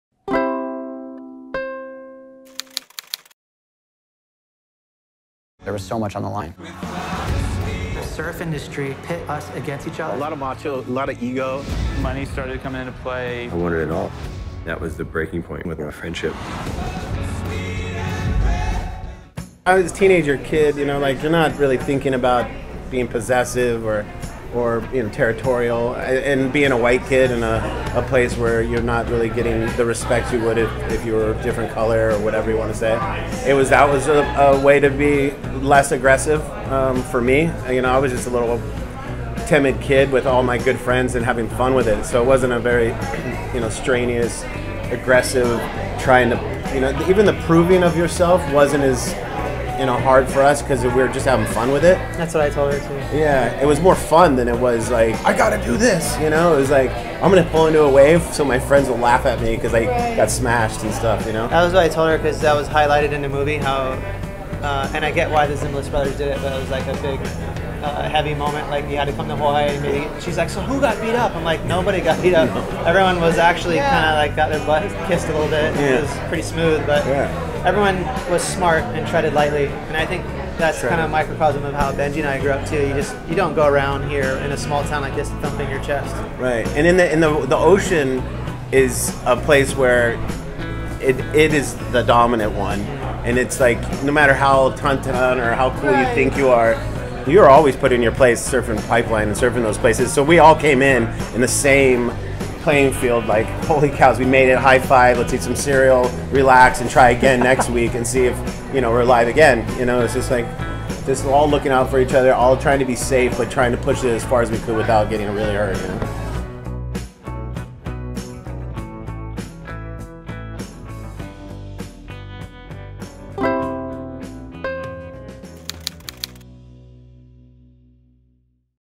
Hawaii surfers featured in HBO documentary 'Momentum Generation'
The "Momentum Generation," features Hawaii surfers who redefined surfing.